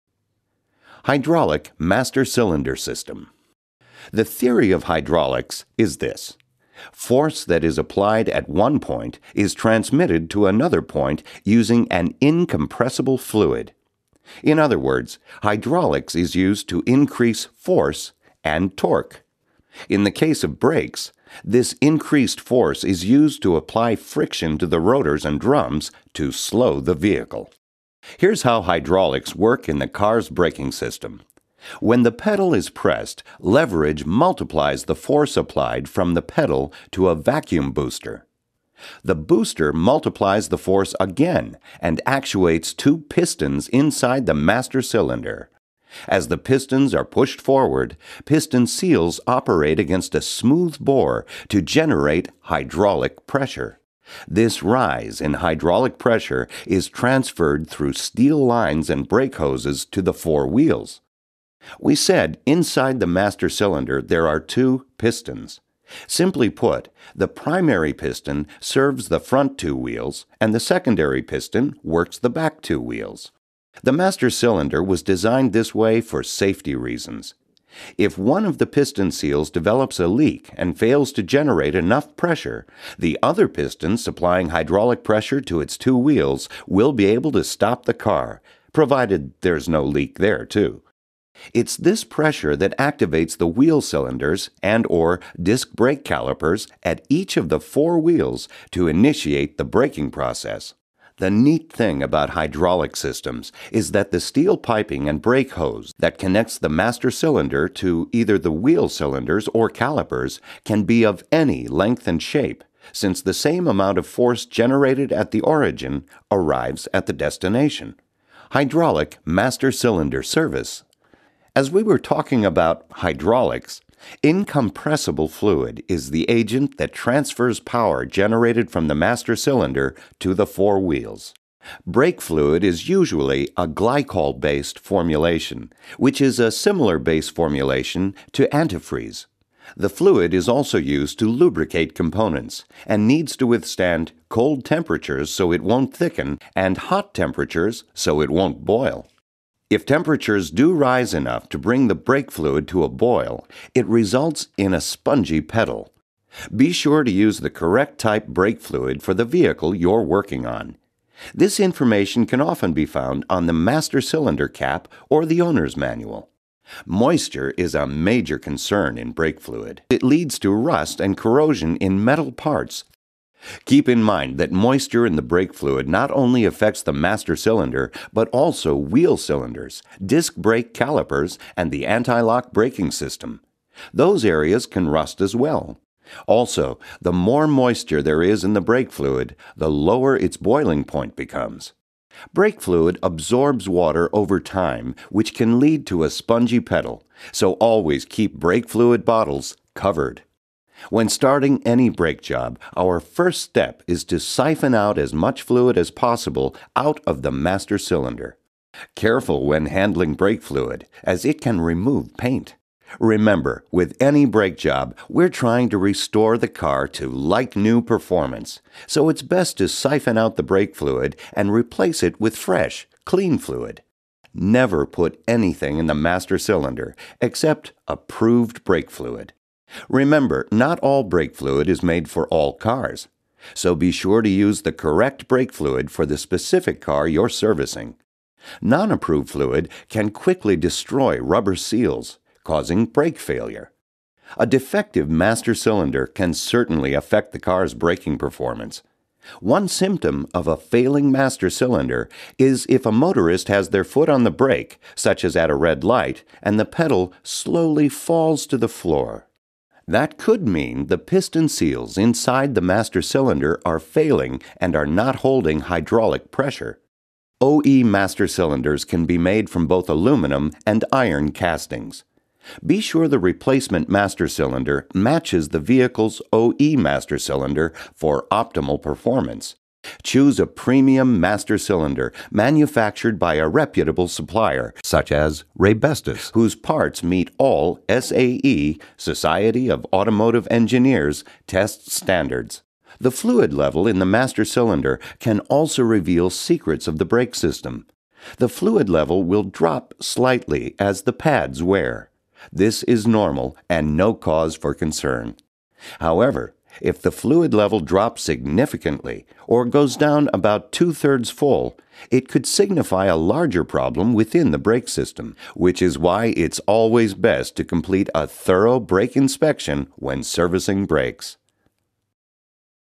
Hydraulic Brakes
this is a great training video showing the basic operation of a hydraulic brake system